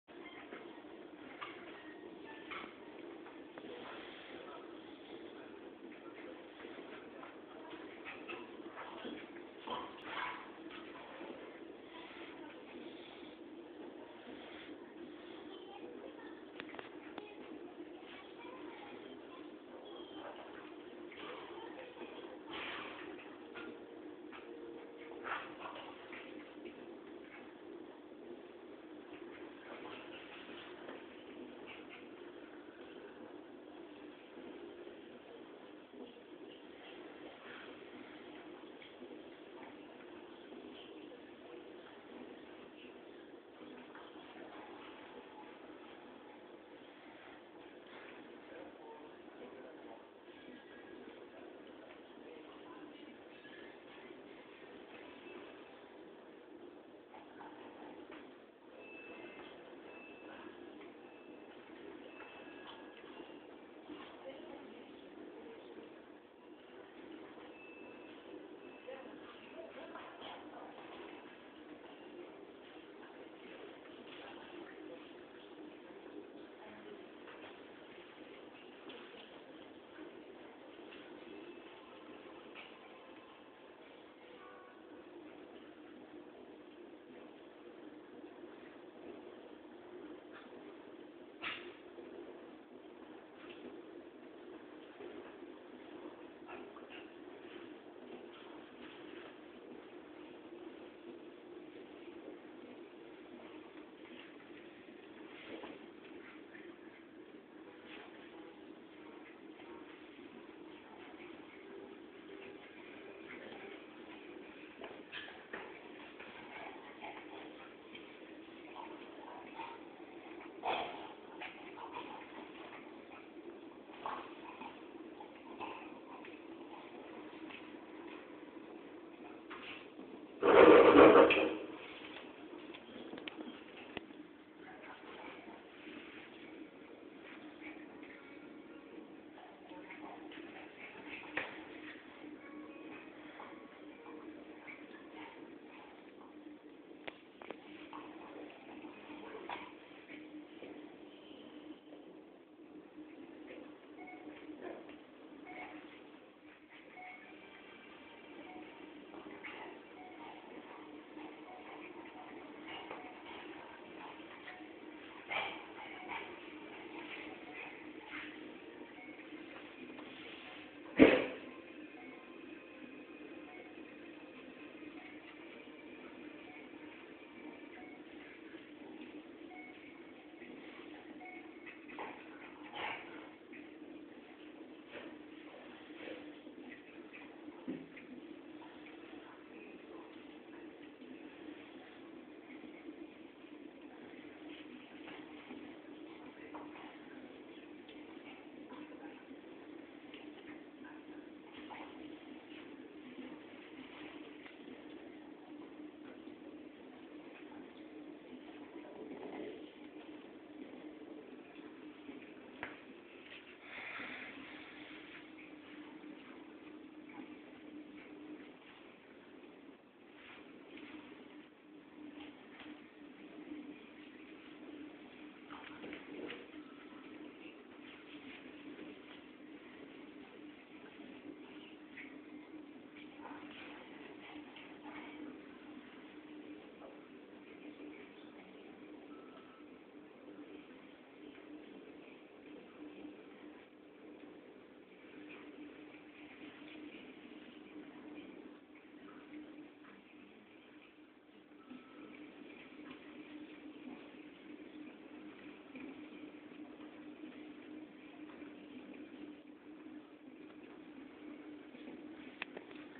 labrador 10 months and maltese 3 months play